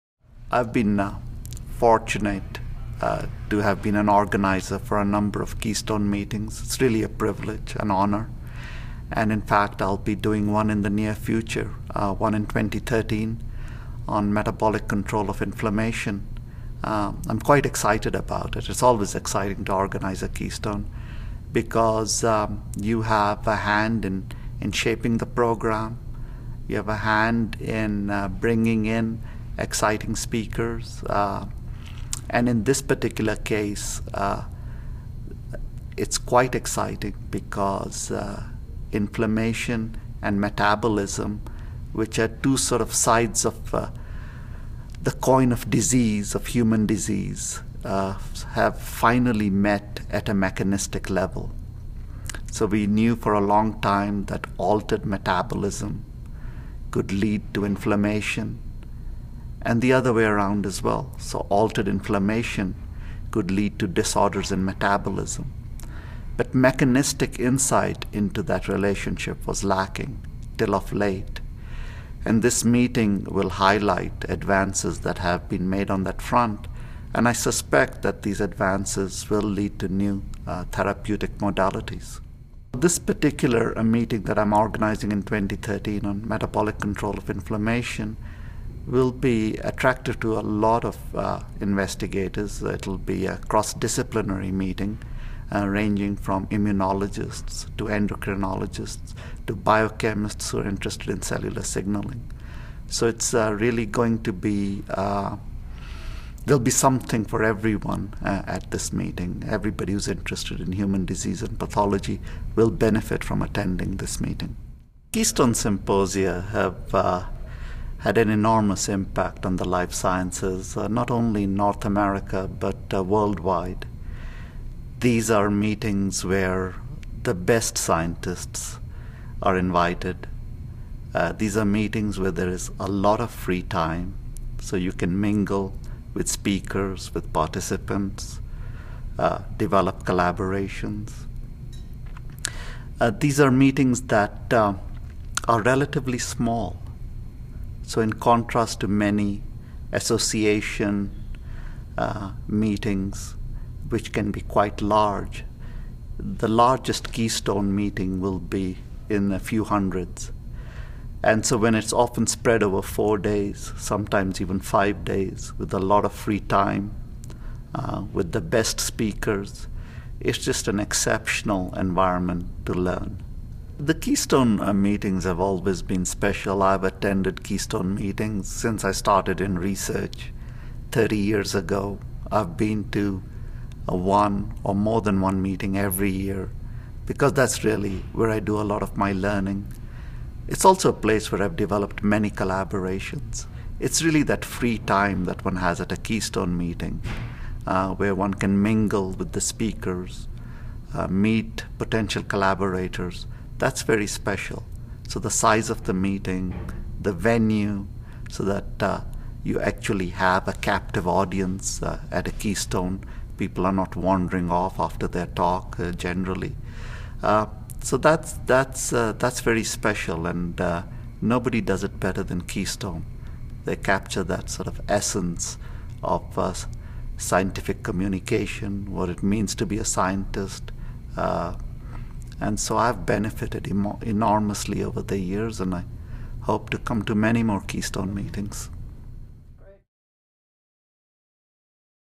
Vishva Dixit: Metabolic Control of Inflammation & Immunity
Vishva M. Dixit, lead scientific organizer of Keystone Symposia's meeting on Metabolic Control of Inflammation and Immunity, discusses the upcoming meeting which takes place January 21-26, 2013 in Breckenridge, Colorado, USA. for more information.